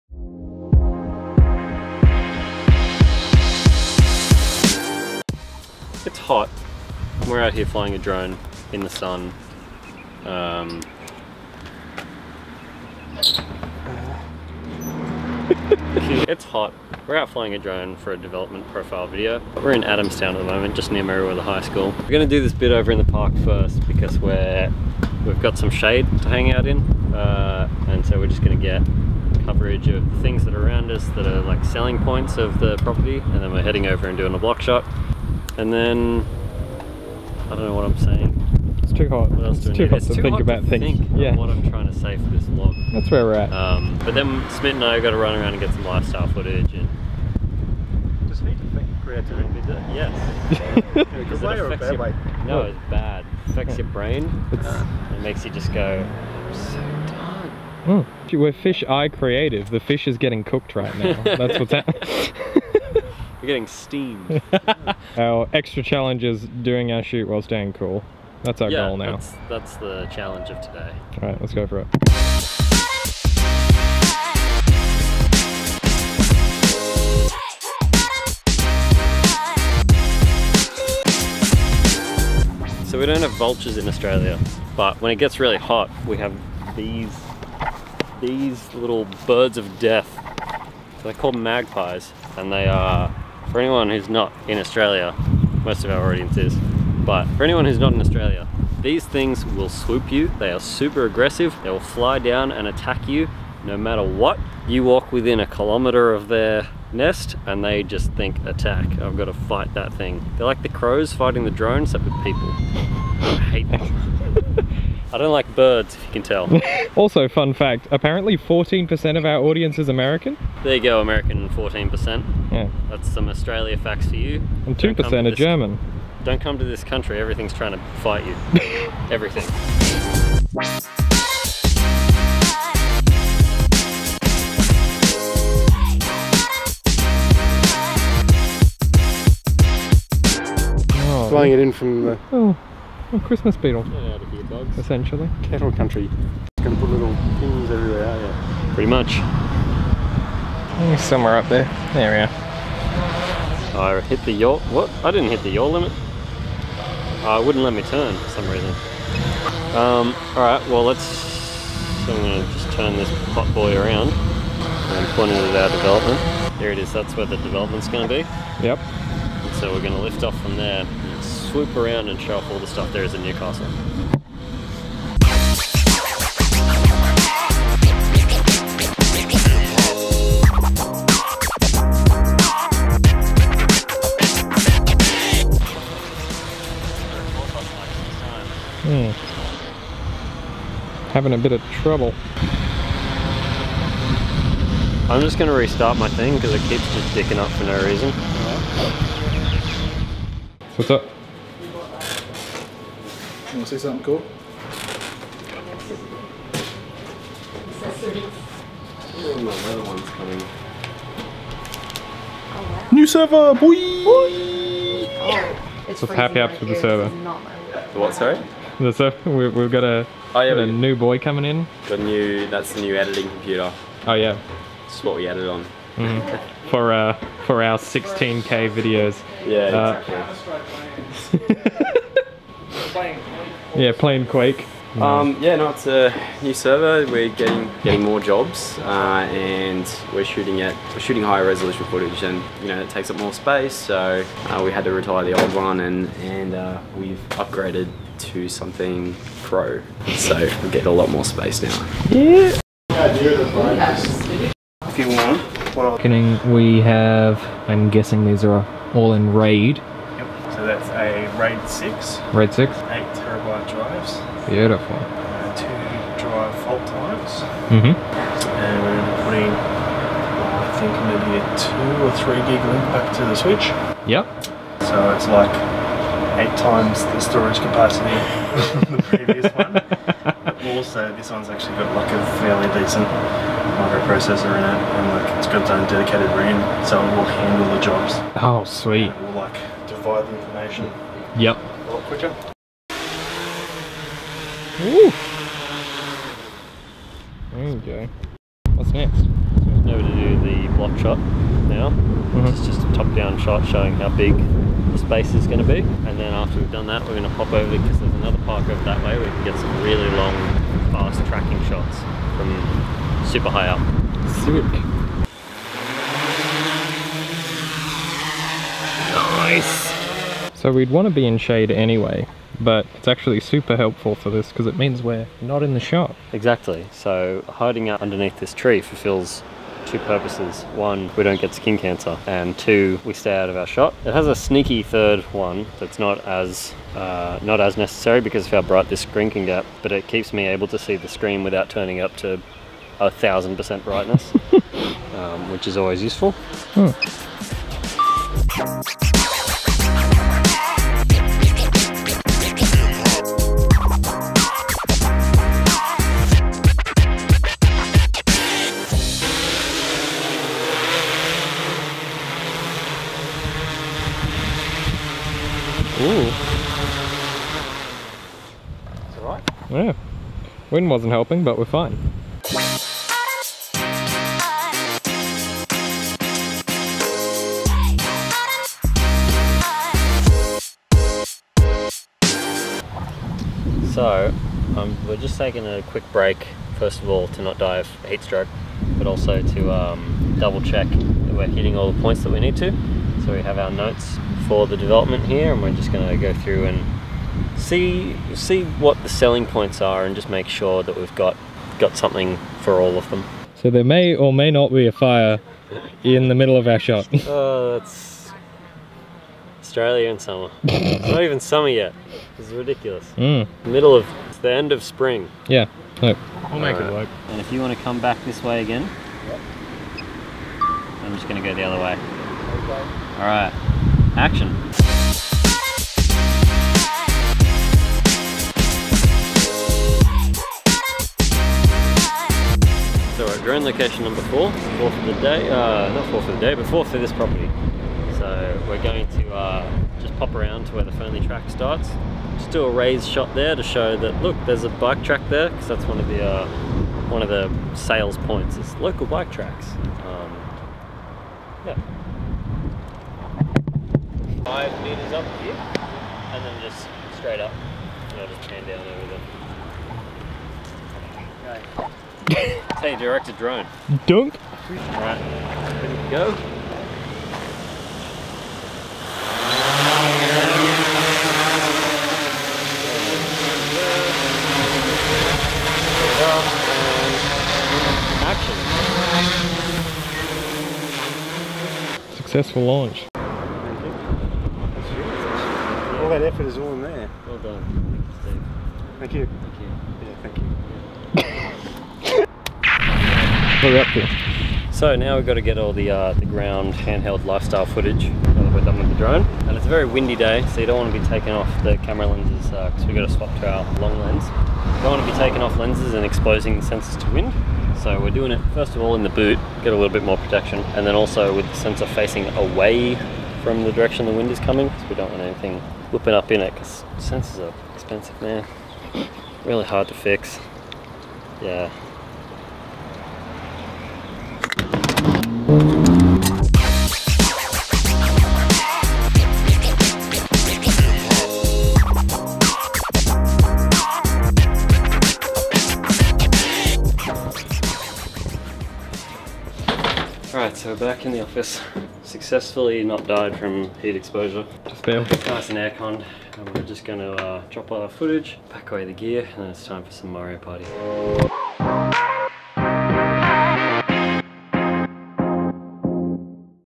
The Development Demo | Fisheye Vlogs
Dan's down at Adamstown to get his hype on for a new development property, and something new's up at HQ!